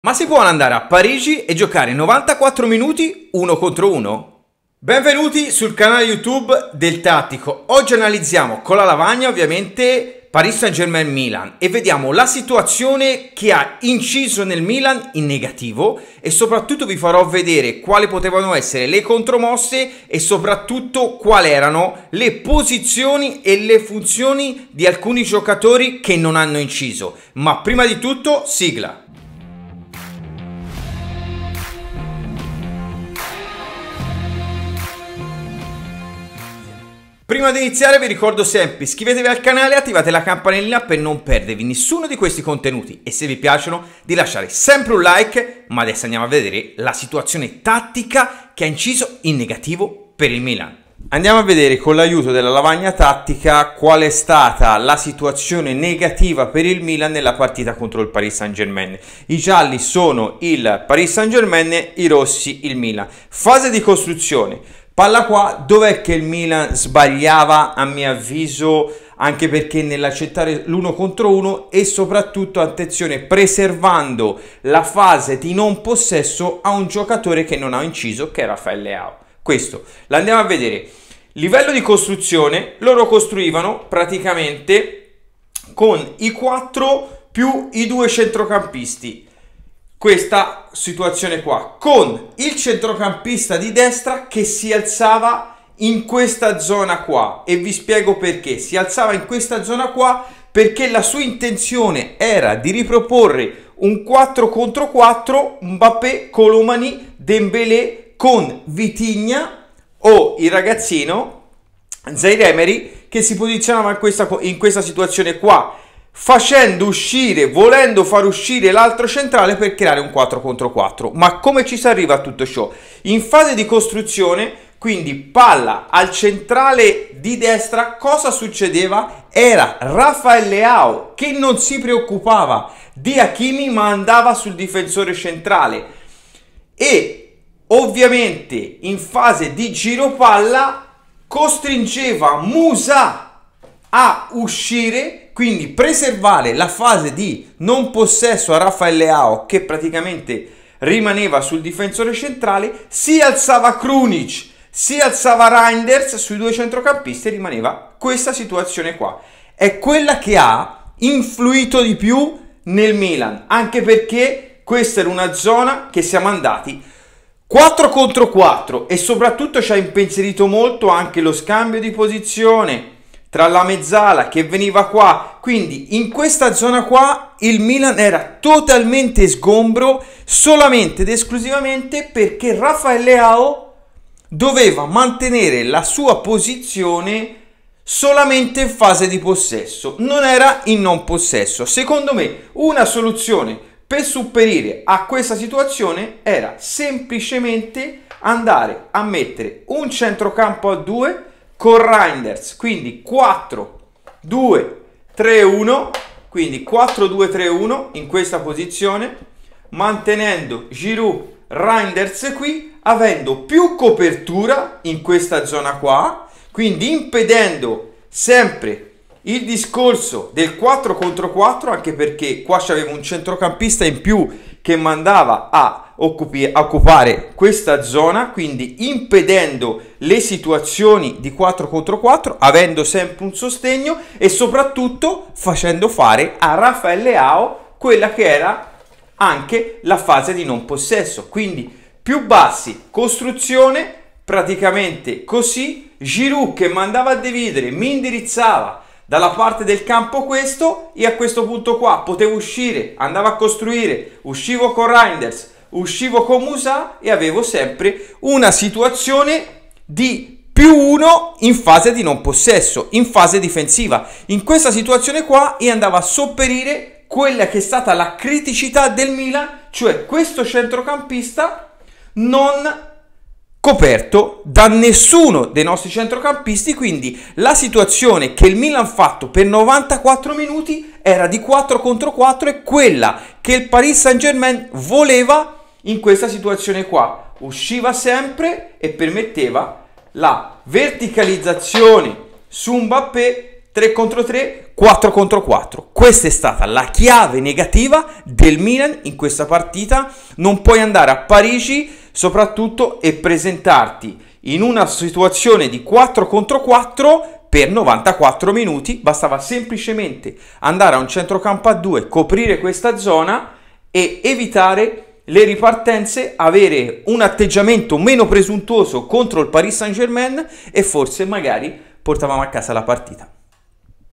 PSG-MILAN LA CHIAVE TATTICA IN NEGATIVO PER IL MILAN!
Ho analizzato PSG.MILAN trovando le difficolta del Milan e sopratutto una possibile soluzione tattica,perchè non e' stato cambiato in corsa e sopratutto perché 1vs 1 per 94 minuti a Parigi?!